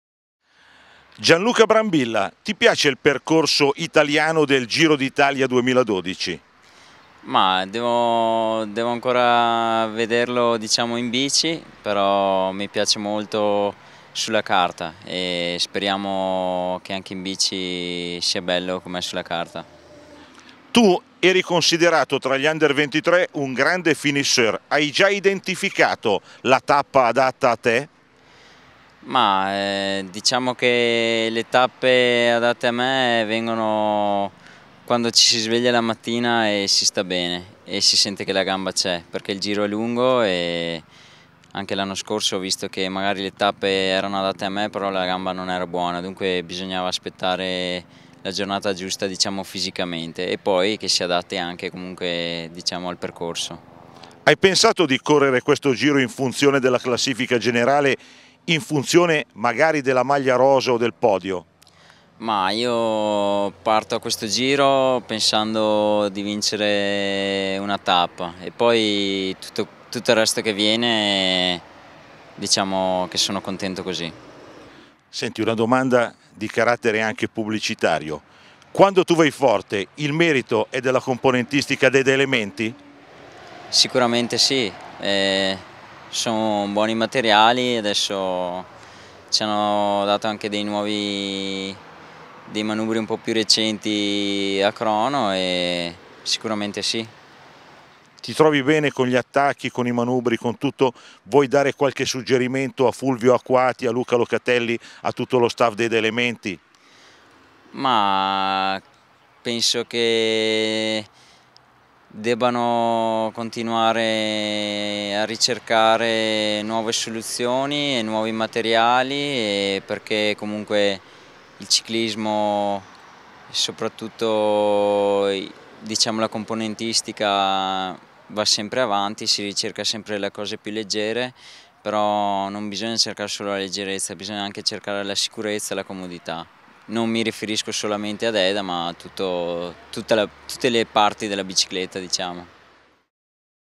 Giro d'Italia 2012 -- intervista a Gianluca Brambilla
Intervista a Gianluca Brambilla della Colnago -CSF inox.
Ad opera di Alessandro brambilla